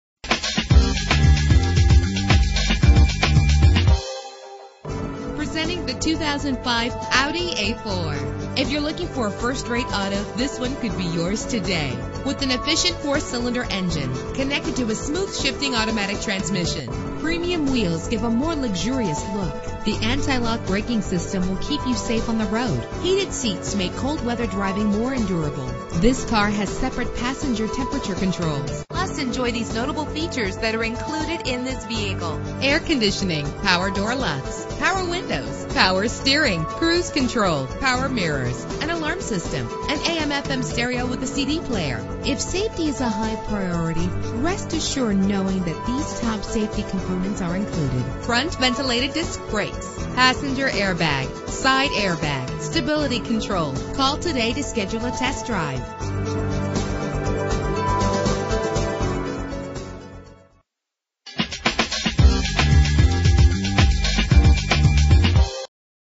2005 Audi A4 - Lynnwood WA
Year: 2005
Make: Audi
Model: A4
Trim: 1.8T
Engine: 1.8 liter inline 4 cylinder 20 valve
Transmission: Automatic CVT
Color: Black
Mileage: 51229
Address:
16601 Highway 99